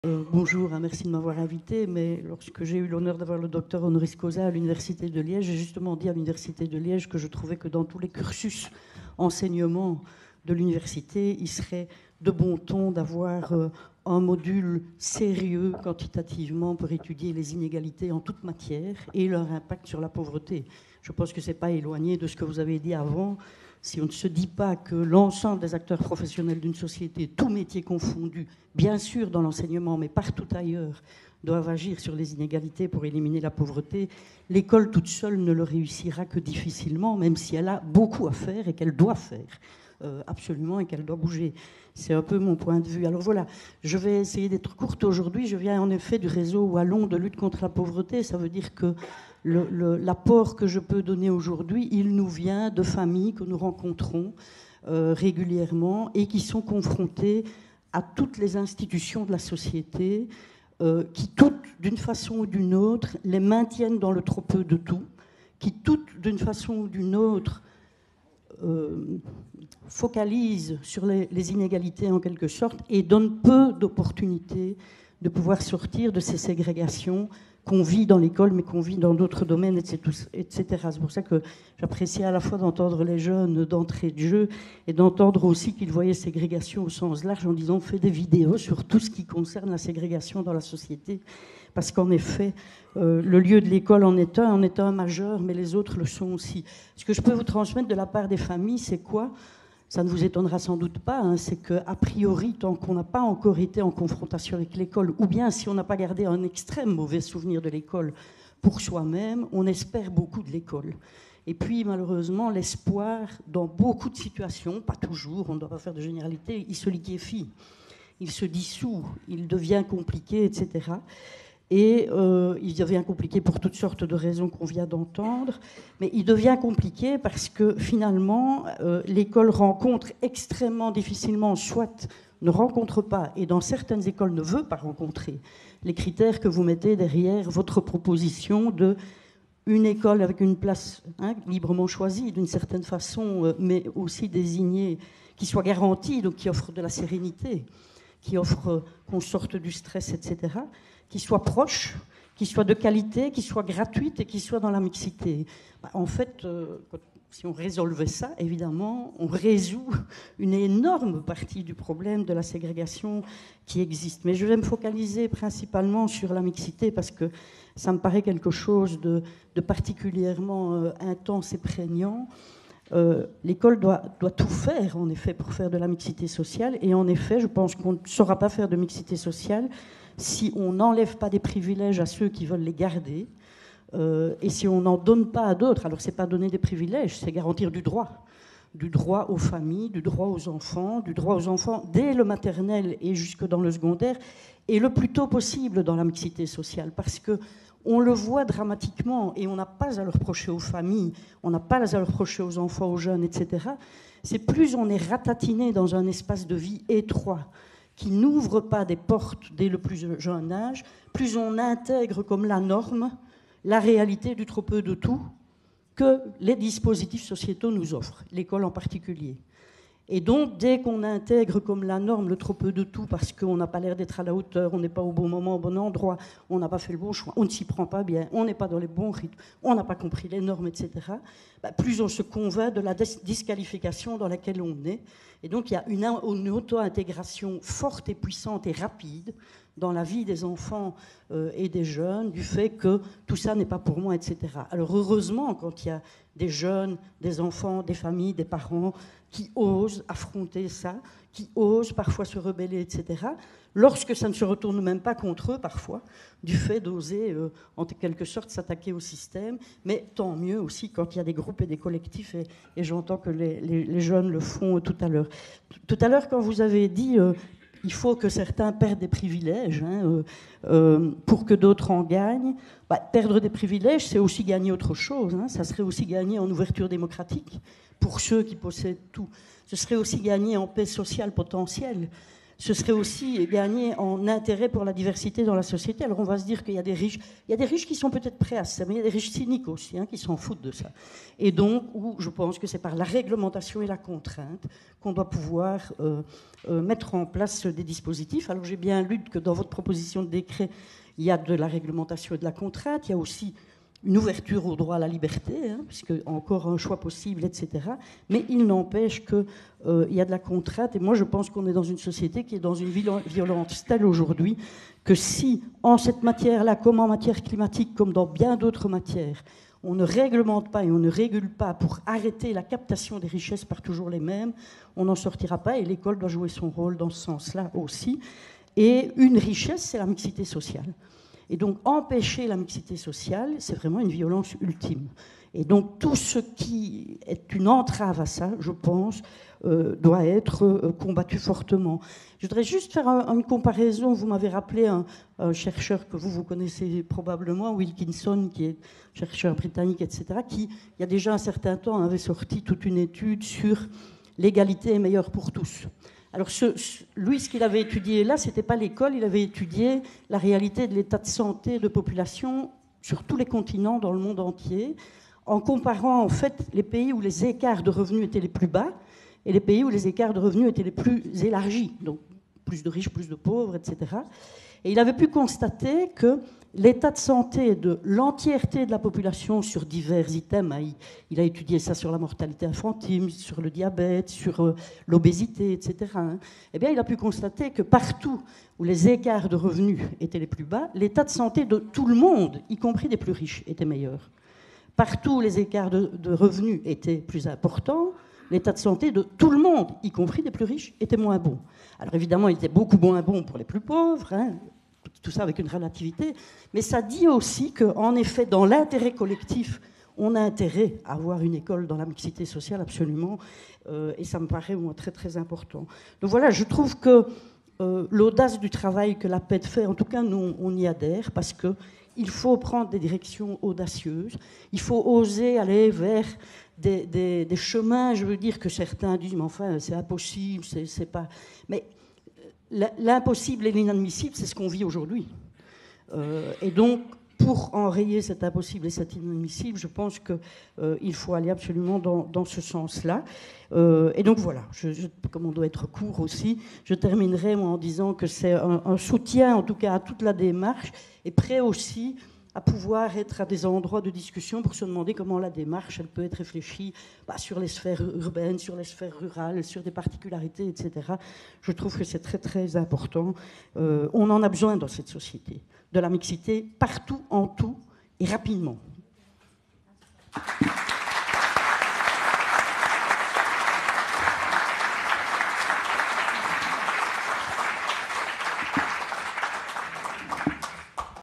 Christine Mahy soutient l'initiative “une place dans un bonne école pour chaque enfant"
Intervention de Christine Mahy, présidente du Réseau wallon de Lutte contre la Pauvreté (RWLP), lors du colloque organisé par l'Aped, le 22 avril 2023 à Louvain.